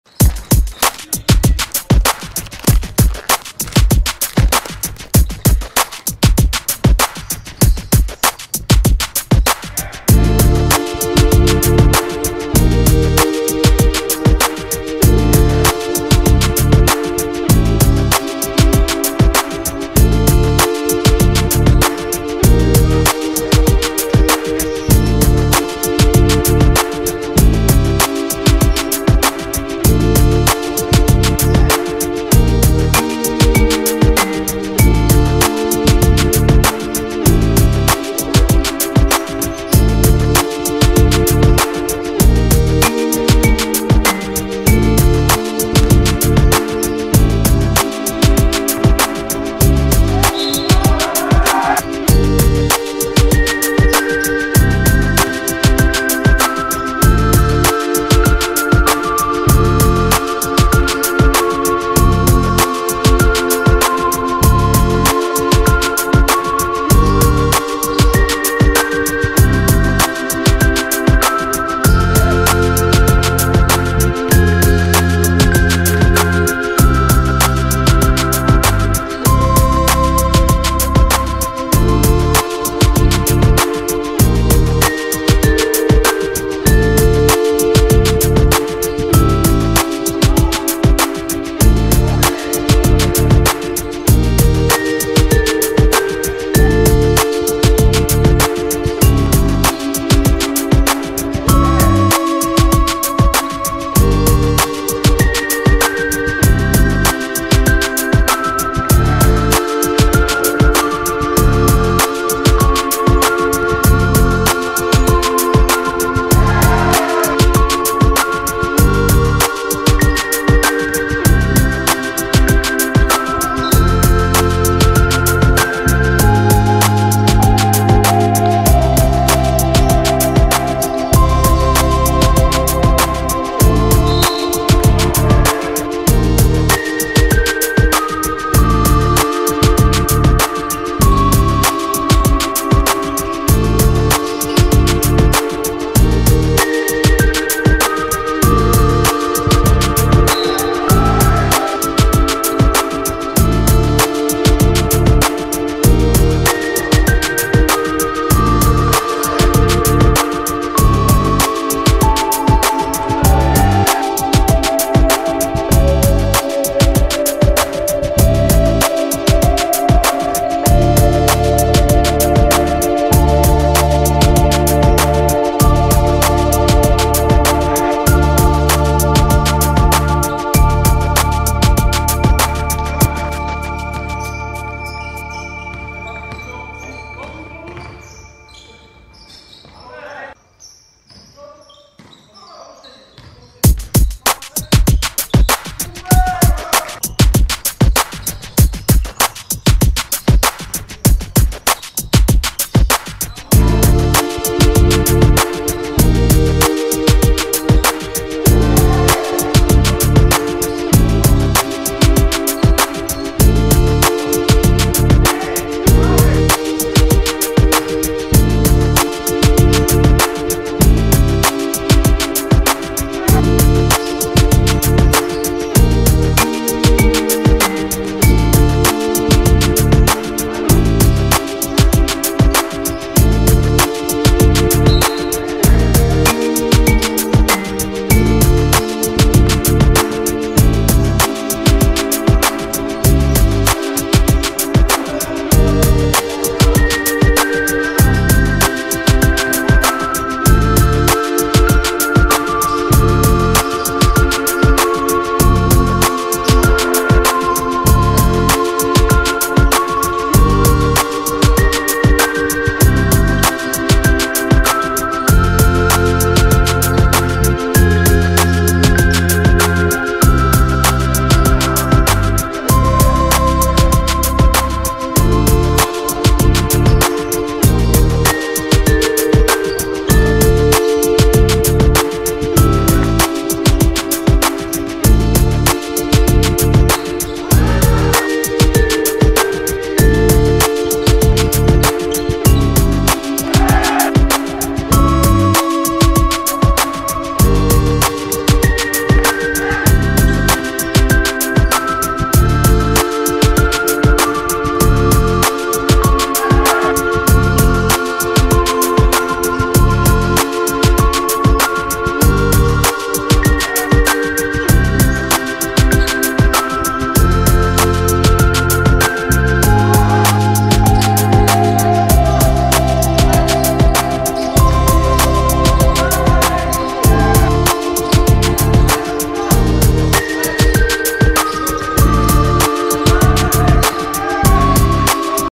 01 10 Indiana vs Los Angeles – Draft Liga Niedziela D1 Jesień 2023
INDIANA 79:83 LOS ANGELES
M.Fiziar 17pkt 10zb 4ast
A.Klimkiewicz 22pkt 3ast 5stl